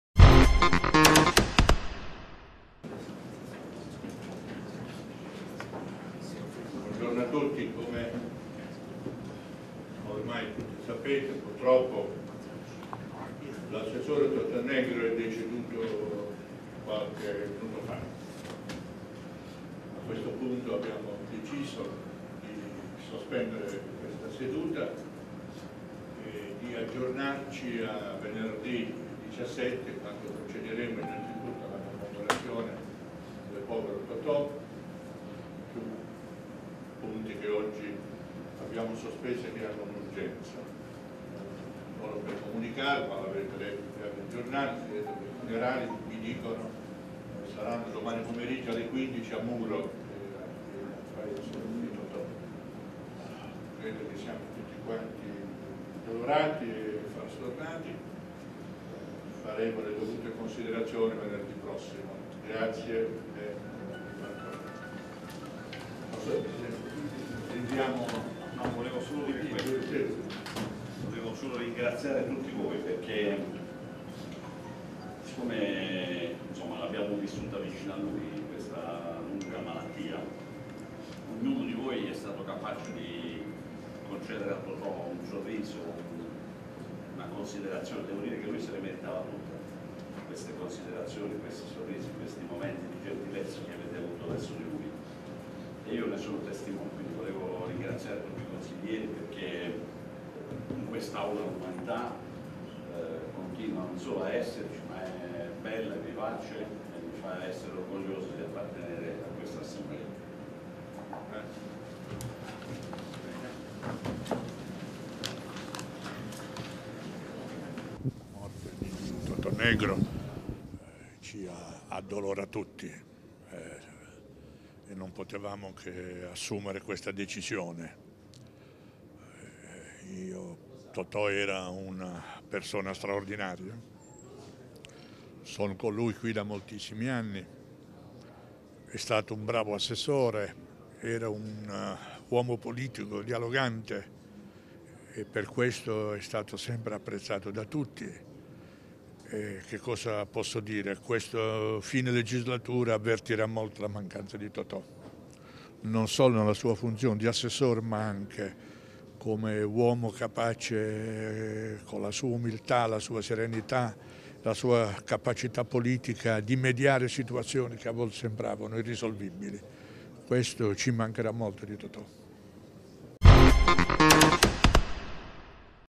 Regione Puglia, seduta rinviata é morto l'assessore Welfare Salvatore Negro
E' venuto a mancare questa mattina
all'età di 64 anni Salvatore Negro, assessore al Welfare della Regione Puglia e consigliere regionale eletto nelle fila dei Popolari.  Nato il 2 gennaio 1953 a Muro Leccese (Lecce), Negro è stato sindaco della sua città per due mandati, dal 1995 al 2004. E' stato poi impegnato come vicepresidente nella Provincia di Lecce, fino al 2009.  Nel 2010 è stato eletto per la prima volta nel Consiglio regionale nella lista Unione di Centro. È stato capogruppo dell'Udc in consiglio regionale e componente della terza Commissione consiliare (Servizi sociali) e della quarta (Sviluppo economico). Poi la riconferma in Consiglio e la nomina ad assessore al Welfare nella giunta regionale guidata da Michele Emiliano. Tra le altre cariche rivestite quella di componente del Consiglio dell'Ordine degli architetti di Lecce (è stato vicepresidente dal 1989 al 1992), del direttivo regionale dell'Anci, della commissione regionale Beni culturali e del Consiglio nazionale dell'Upi.